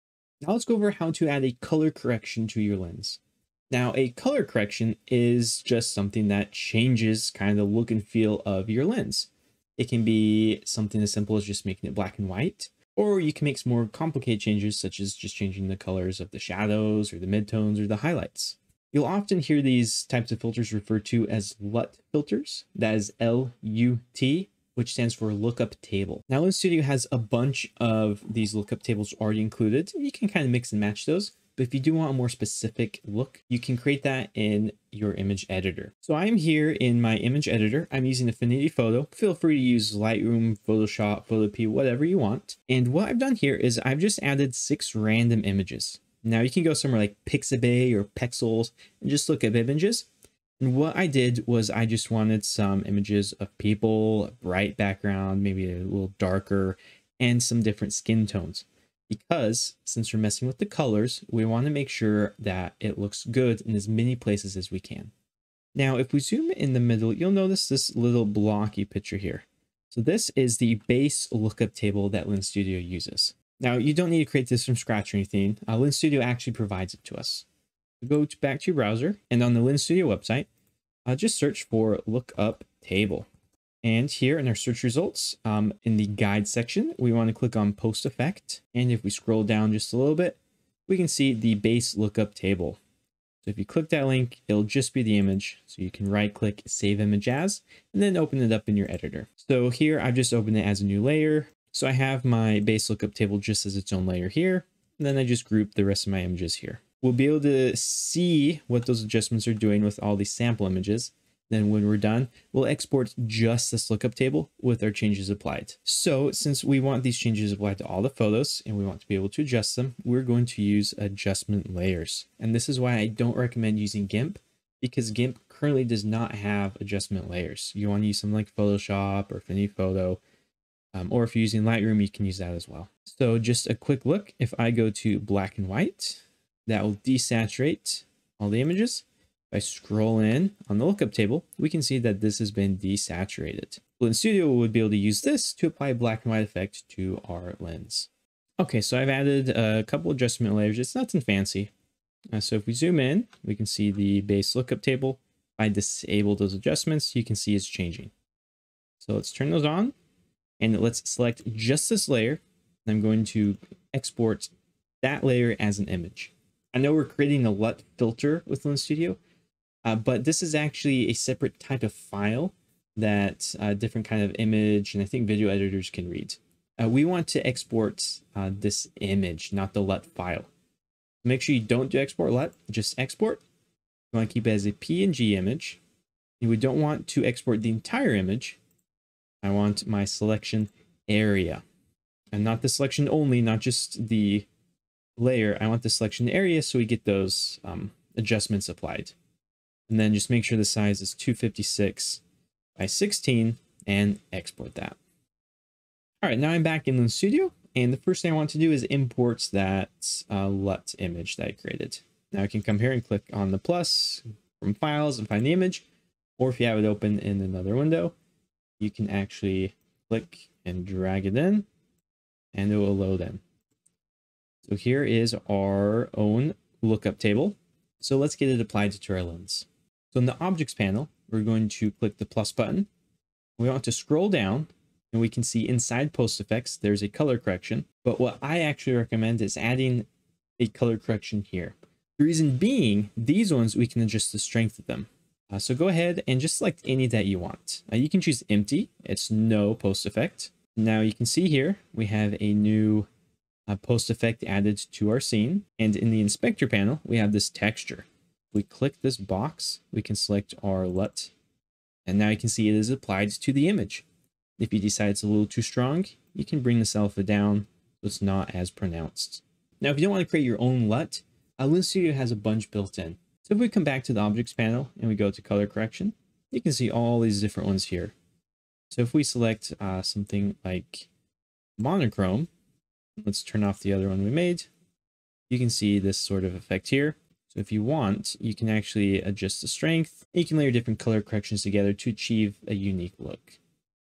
How to make free Snapchat Lenses - #4 Color corrections and LUTs - Lens Studio tutorial
Add a color correction or LUT to enhance the feel of your lens! A color correction is a nice way to enhance the look and feel of your lens. Color corrections are controlled by lookup tables, or LUTs for short.